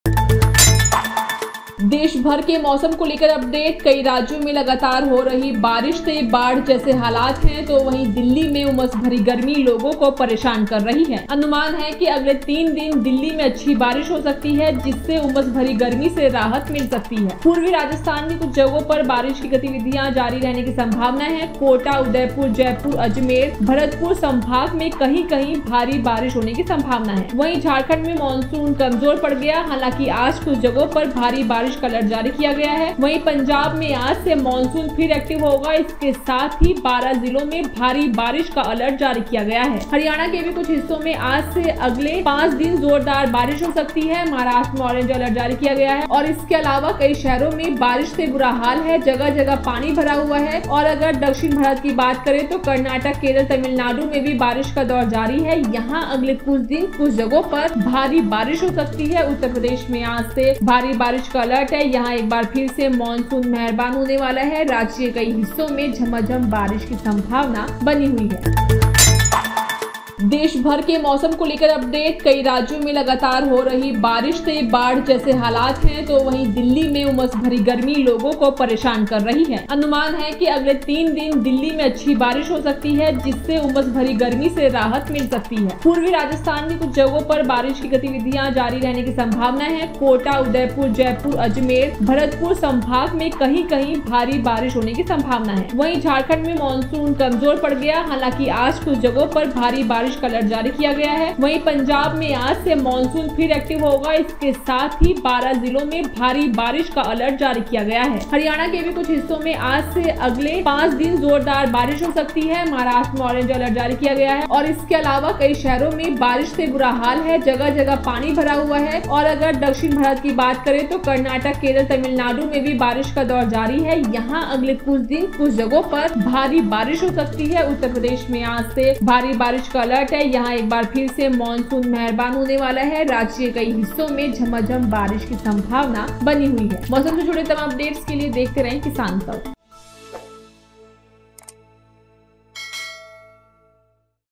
breakingnews: Monsoon Rain Flood Update | इन राज्यों में Heavy Rain Alert | Weather
कई राज्यों में लगातार हो रही बारिश से बाढ़ जैसे हालात हैं... तो वहीं दिल्ली में उमस भरी गर्मी लोगों को परेशान कर रही है. अनुमान है कि अगले तीन दिन दिल्ली में अच्छी बारिश हो सकती है...जिससे उमस भरी गर्मी से राहत मिल सकती है. पूर्वी राजस्थान के कुछ स्थानों पर  बारिश की गतिविधियां जारी रहने की संभावना है कोटा, उदयपुर, जयपुर, अजमेर तथा भरतपुर संभाग में कहीं-कहीं भारी बारिश होने की संभावना है। झारखंड में मॉनसून के कमजोर पड़ गया है. हालांकि आज कुछ जगहों पर भारी बारिश का अलर्ट जारी किया गया है